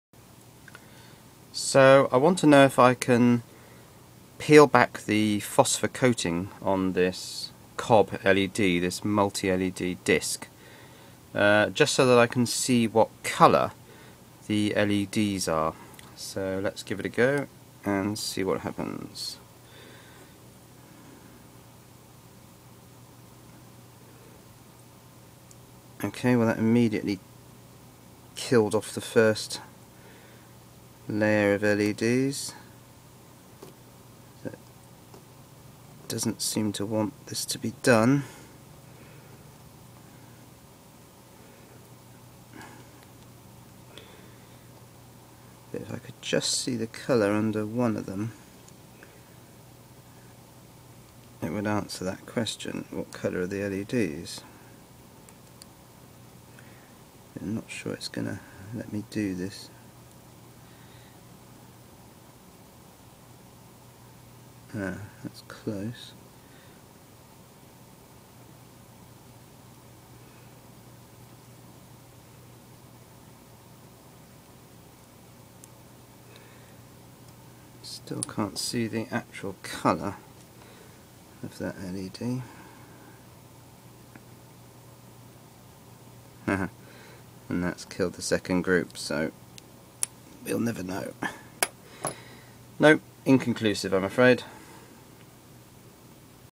What colour are the LEDs underneath the phosphor of this COB LED?
This 5W COB (Chip on Board) LED has a circular layer of phosphor impregnated jelly poured over the array of LEDs. It's very obviously yellow, so it's possible the LEDs are blue or UV. Is it possible to peel back the phosphor and reveal the LED colour?